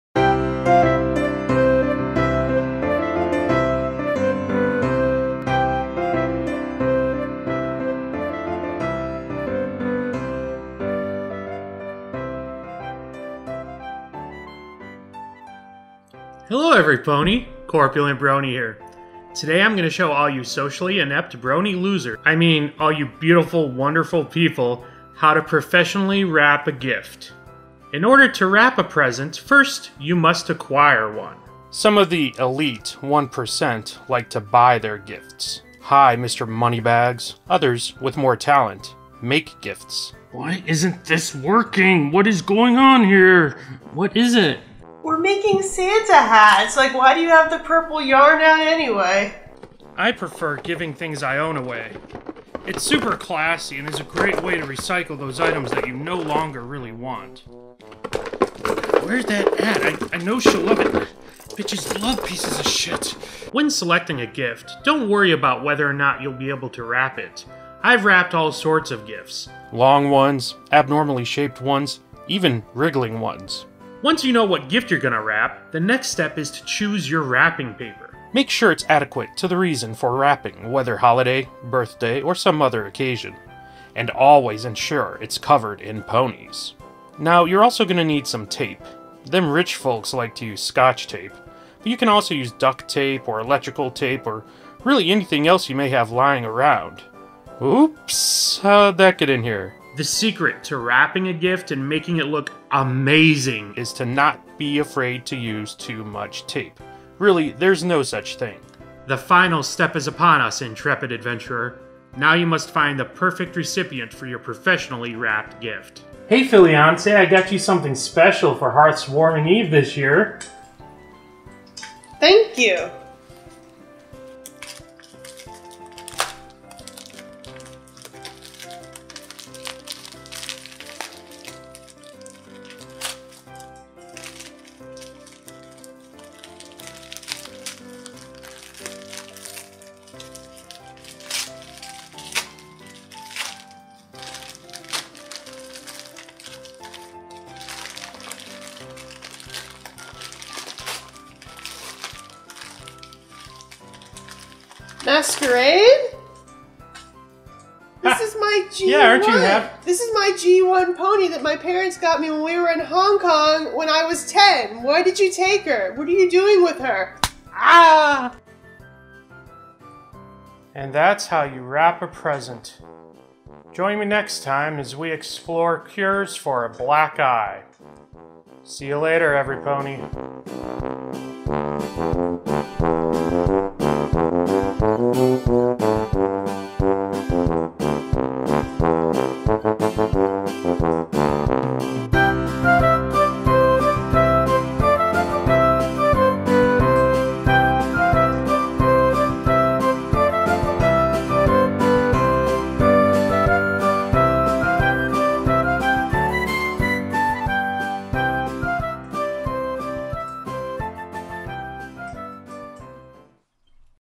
How To Wrap a Present Corpulently
The holidays are upon us, and Corpulent Brony will show you the most important steps in wrapping a gift.  From choosing the present, to picking the wrapping paper, to selecting the best tape to use, all aspects of preparing a present to be given are analyzed in excruciating and Corpulent detail.

MERRY CHRISTMAS AND HAPPY HEARTH'S WARMING EVE ERRYPONEY!

Thanks to Igor at My Little Ties for the Twilight Sparkle tie and tie pin at 0:31

And yes, that is a bag with about $100 worth of dollar coins in it, specifically Ronald Regan

Thanks to PlushWaifus for the large Twilight Sparkle plush seen at 0:50 and 1:18

Thanks to scarf anon (aka CloverMerch) for the /mlp/ Giddy Up! scarf at 1:56

Thanks to Hasbro and its licensed manufacturers for all other My Little Pony products seen throughout the video.